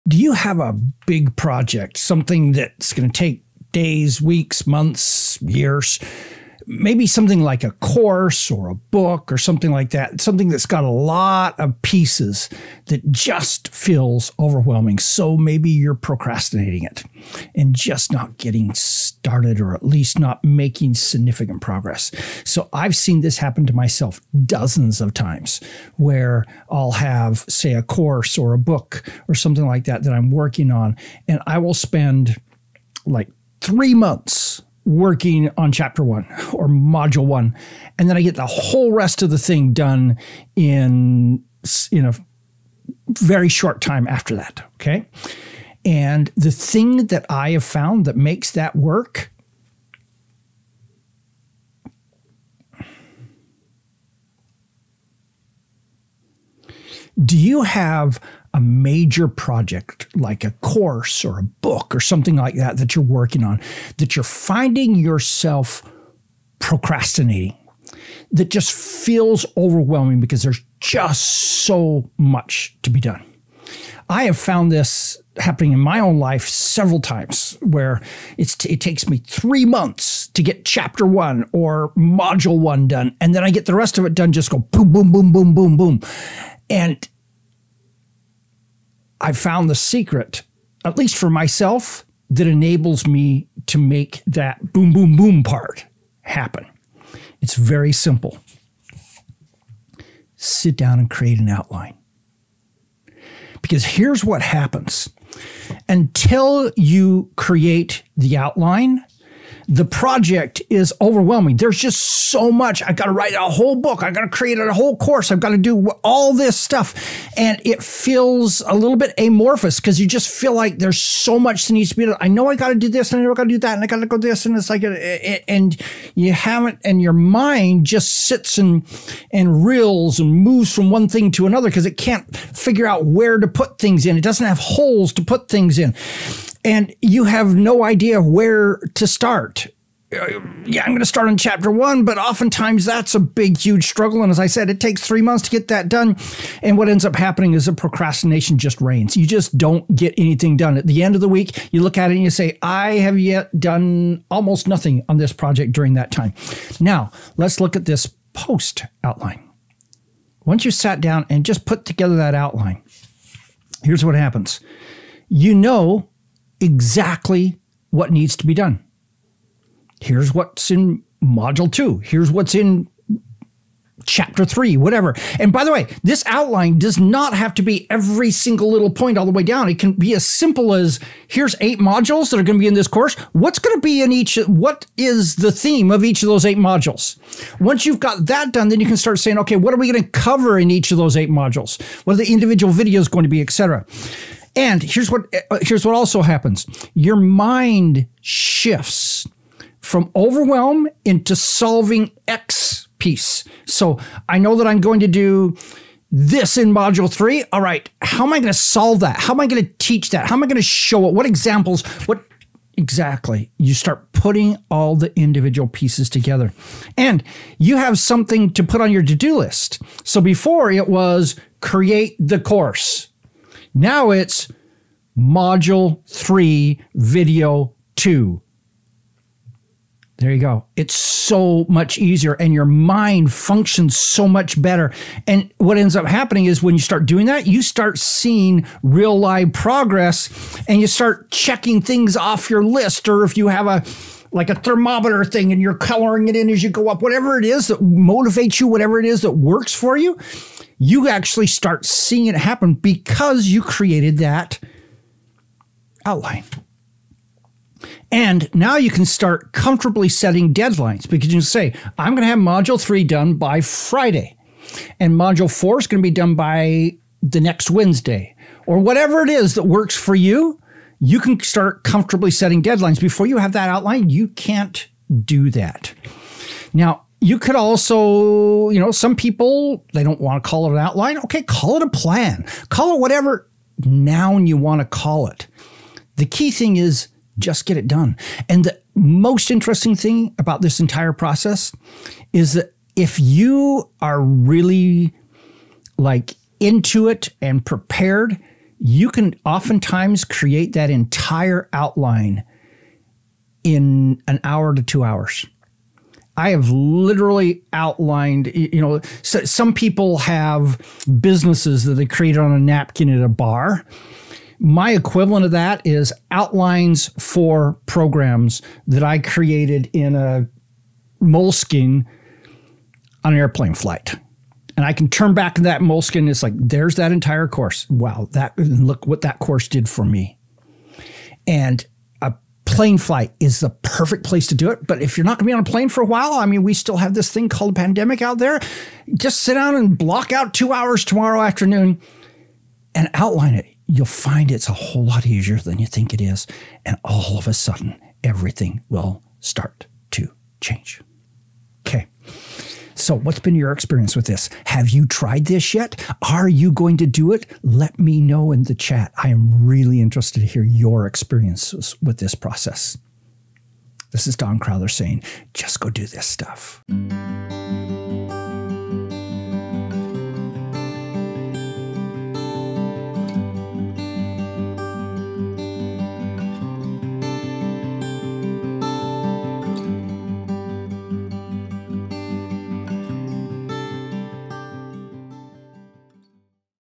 My Productivity Secret: How To Beat Procrastination And Get More Done - Don Crowther
Want to know a huge productivity secret that will help you to stop procrastinating your big projects?

It’s very simple - outline your project.

That’s it. Sit down and outline each key element of your project.

As soon as you do that your productivity will soar because now it shifts from this huge amorphous task lurking out there for you to get done, it’s now a series of simple steps. Steps that can be added to a to-do list. Steps that can easily be completed, then crossed off your list, so you feel like your making progress.

Once you create the outline, then start putting the elements on that outline onto your calendar and to-do lists.

Pretty soon, you’ll find yourself not only making progress, but feeling excited about the progress you’re making.

Watch this video to see exactly how to do it.

Key concepts